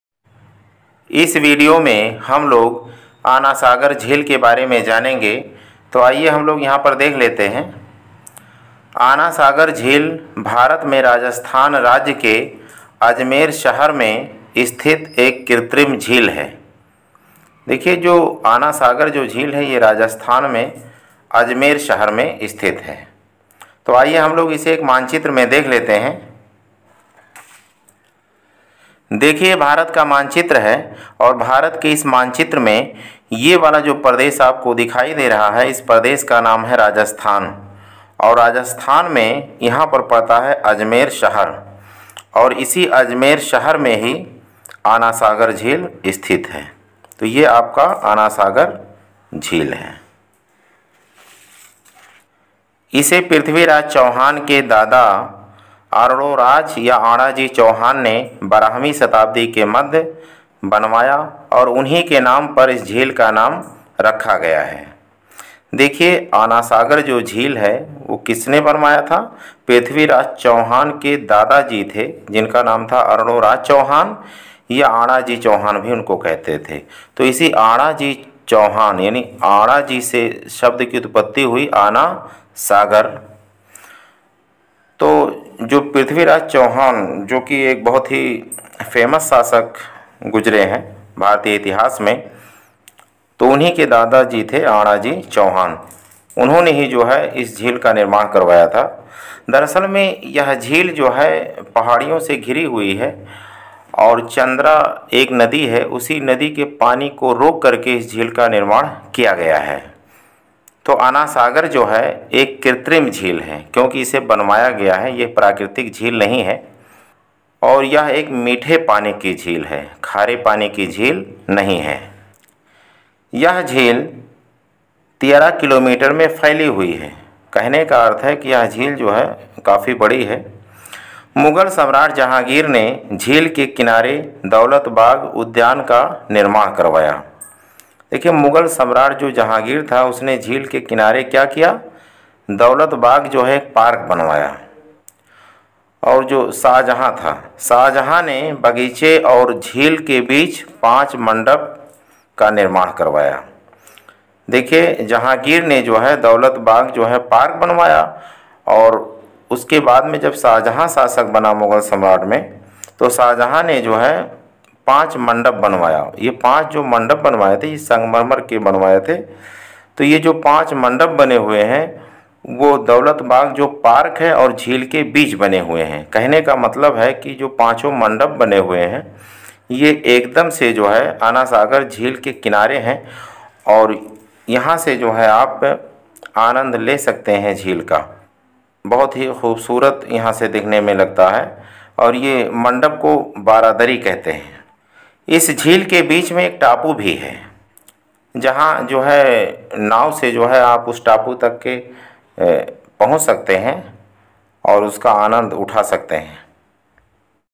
आना सागर झील Ana Sagar Lake
This video has been told about Ana Sagar Lake.इस वीडियो में आना सागर झील के संबंध में बताया गया है is veediyo mein aana saagar jheel ke sambandh mein bataaya gaya hai